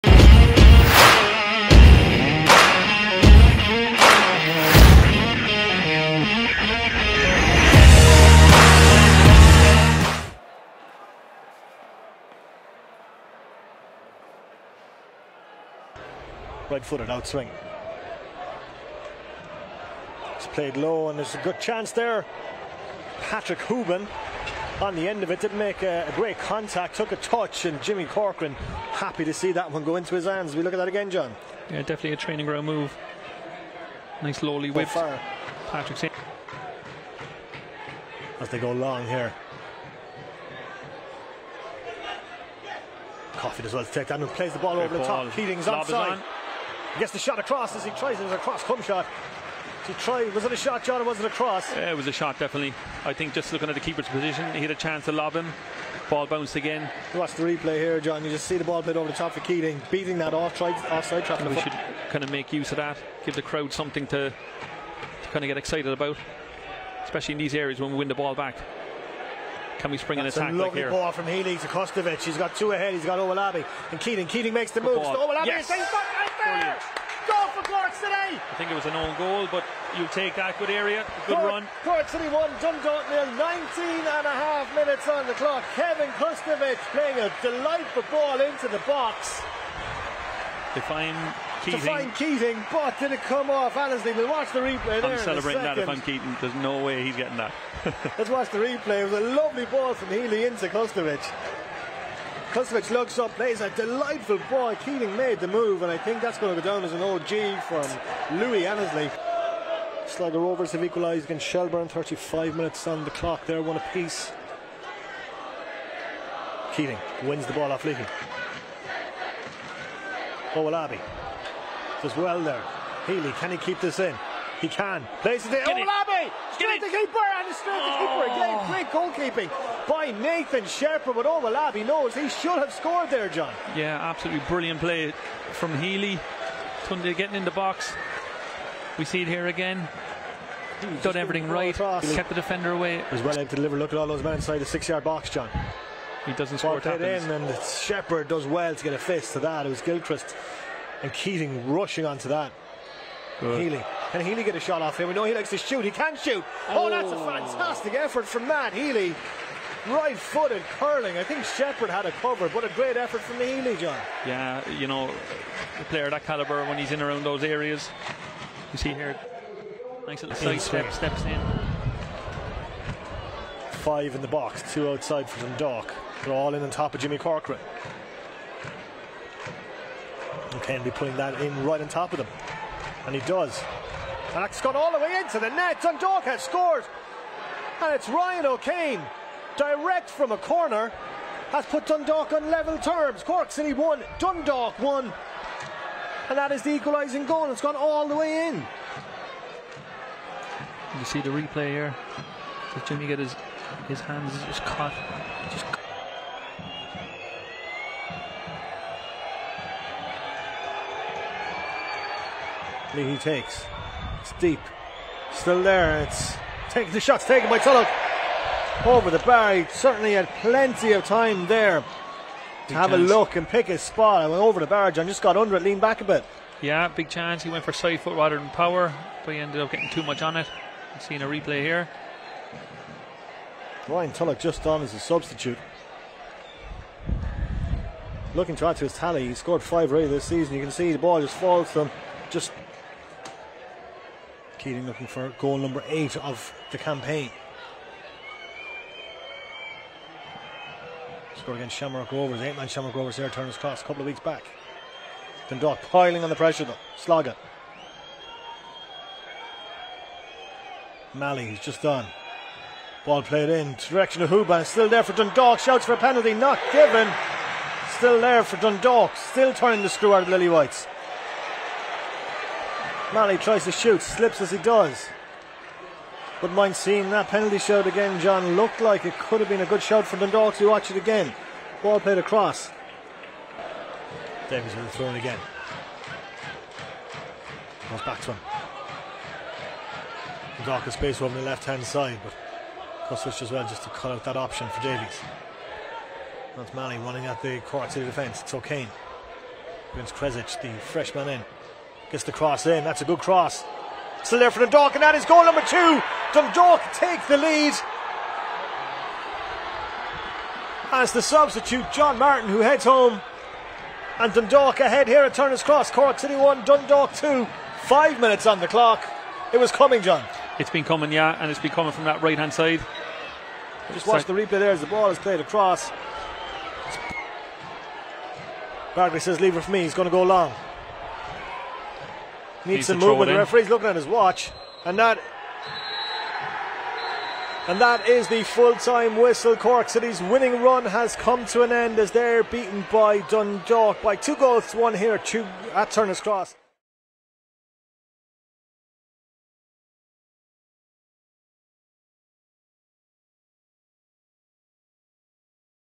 Highlights | Cork City 1-2 Dundalk FC
In association with Nude Foods, watch the best of the action from our 2-1 win over Cork City at Turner's Cross.

⚽︎ League of Ireland
⚽︎ Turner's Cross